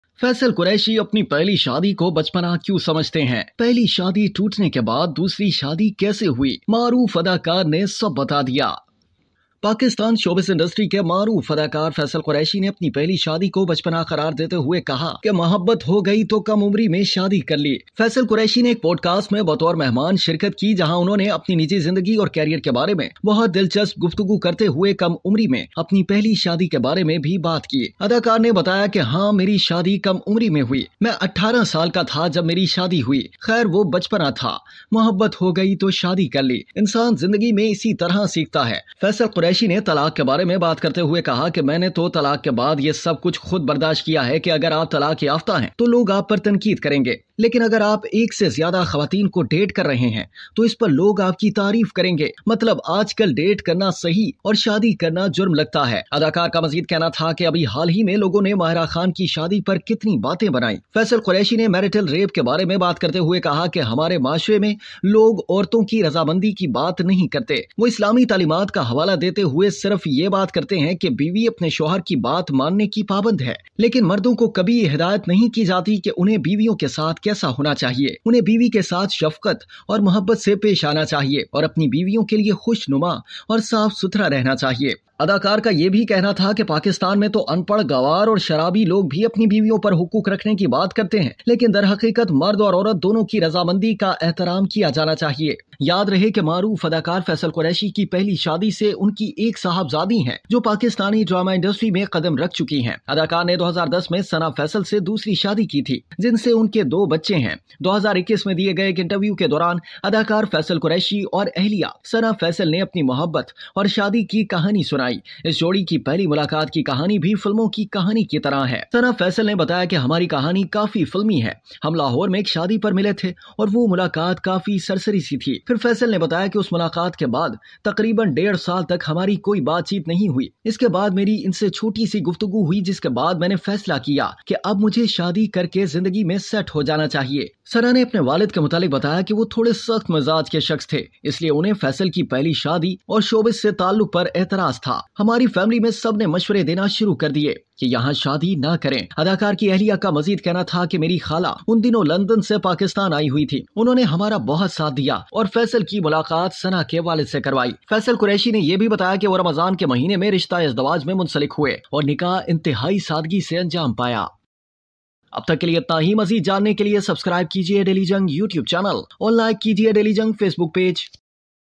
Why does Faysal Quraishi consider his first marriage as 'Childhood'?
Faysal Quraishi, a well-known actor of Pakistan showbiz industry, has described his first marriage as a childhood and said that he got married at a young age when he fell in love.